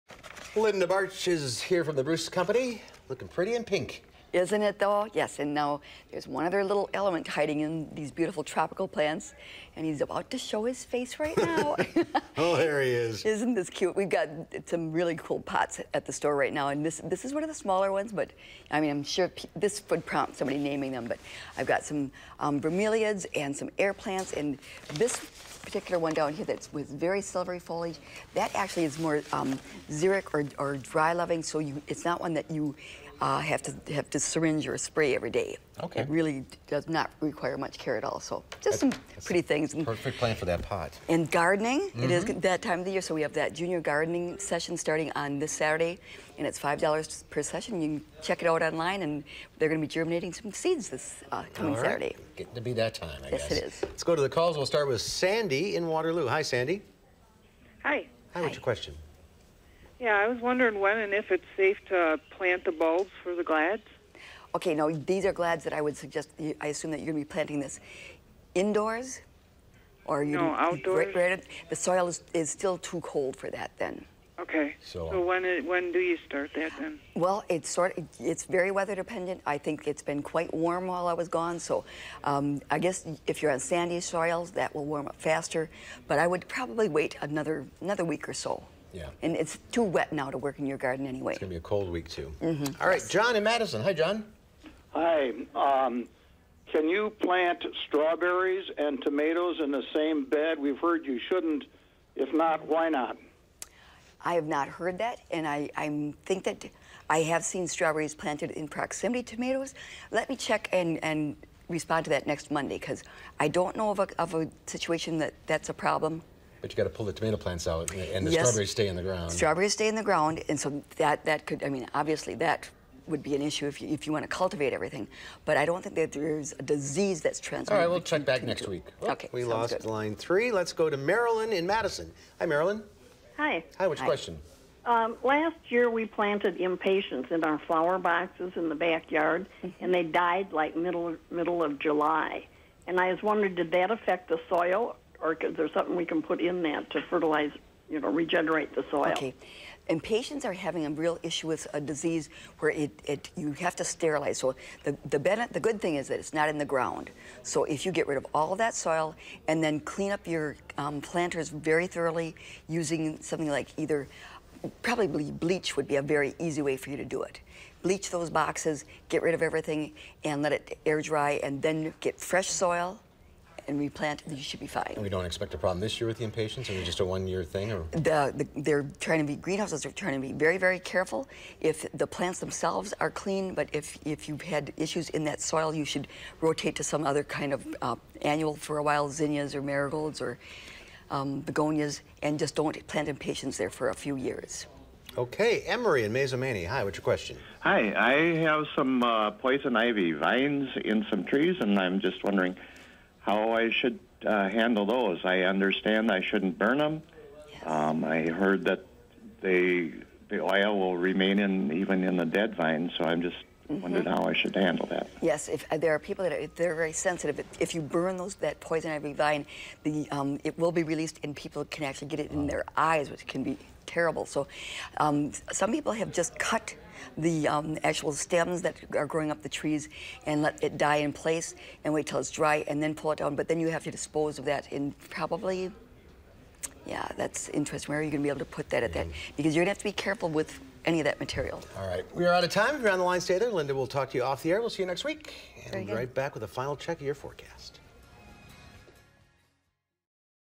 Linda answers your plant and gardening questions
Linda Barcz from the Bruce Company takes your plant questions.